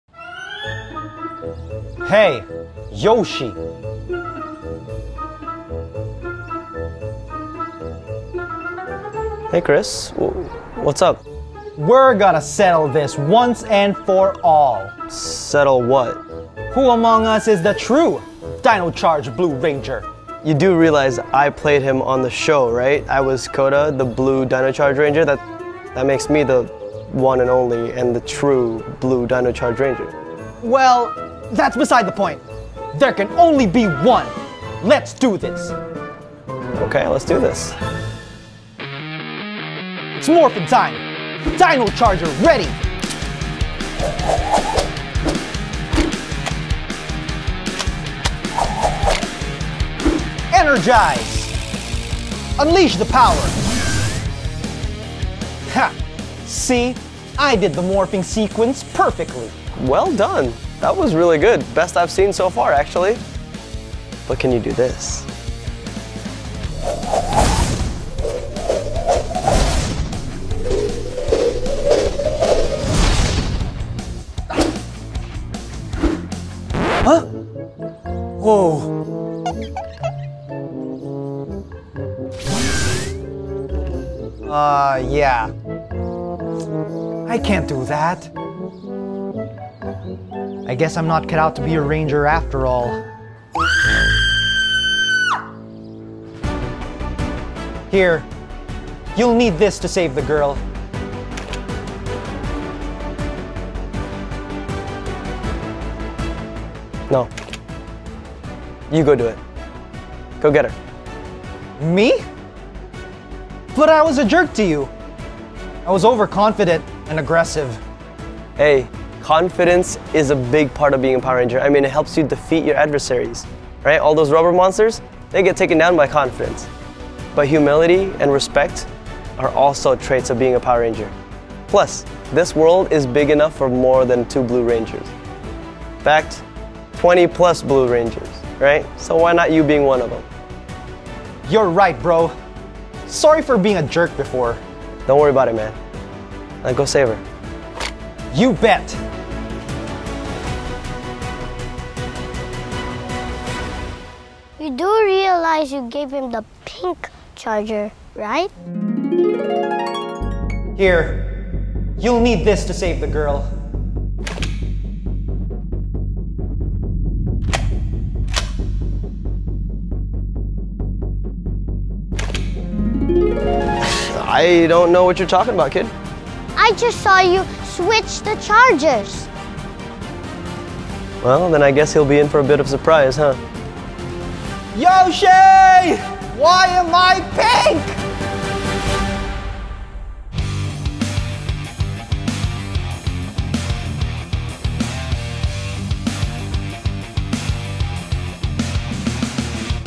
There Can Only Be One - feat. Yoshi Sudarso [FAN FILM] Power Rangers
Here's my newest Fan Film with the one and only Yoshi Sudarso, Koda from Power Rangers Dino Charge and Dino Super Charge!